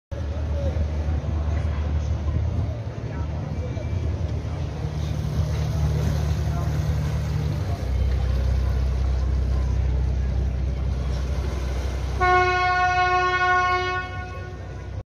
Boats in canals in Europe are like Rickshaws in streets of Pakistan
Canals are like streets in Europe, Boats are like Rikshaws in streets.